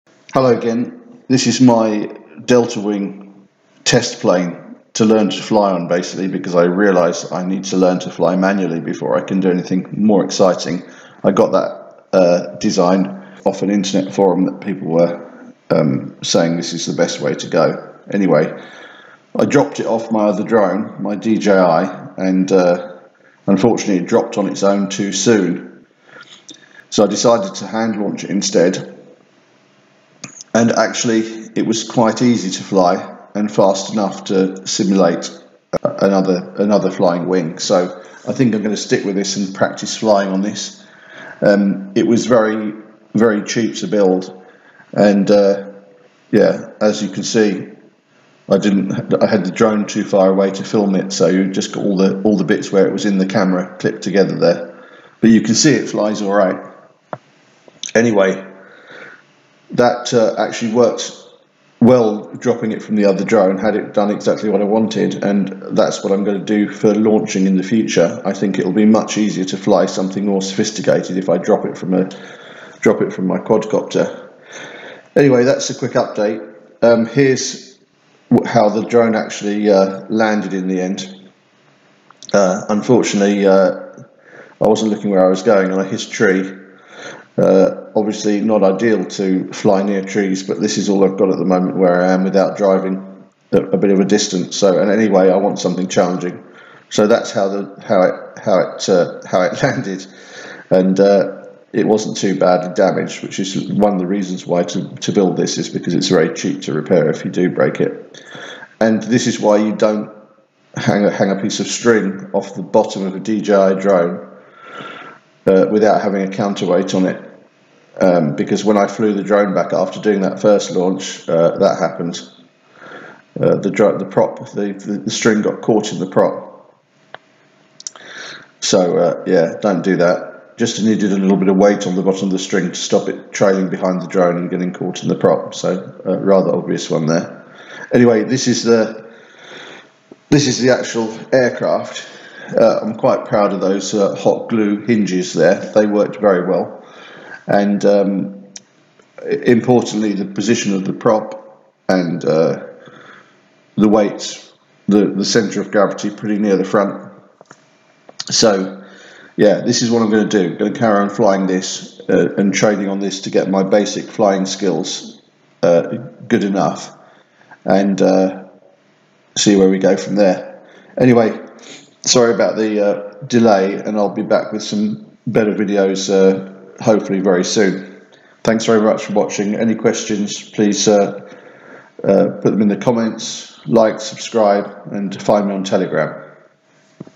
My trainer delta wing for learning manual RC flight launched from DJI Mavic Air
If you're trying to build and fly a UAV, flying wing or any type of fixed wing aircraft using a Pixhawk or any other type of flight controller with mission planner then you need manual flight skills first. If you're building something yourself then you'll be unlikely to be able to fly it on any sort of auto modes until you have it calibrated in manual first and for this you'll need manual flight skills. If you crash a decent model it will be expensive as I discovered quickly. This video shows my solution and also the potential to launch from a drone thus giving you safe launch altitude and time to think, I'm almost certainly going this route with my final model it will also leave the UAV with maximum battery having not burned a load getting altitude. Automating a drone to carry the UAV to a really safe launch altitude is actually simpler than a hand launch and what's more you can do it all alone. Anyway I'll be back soon with some more updates. Please ask questions in the comments.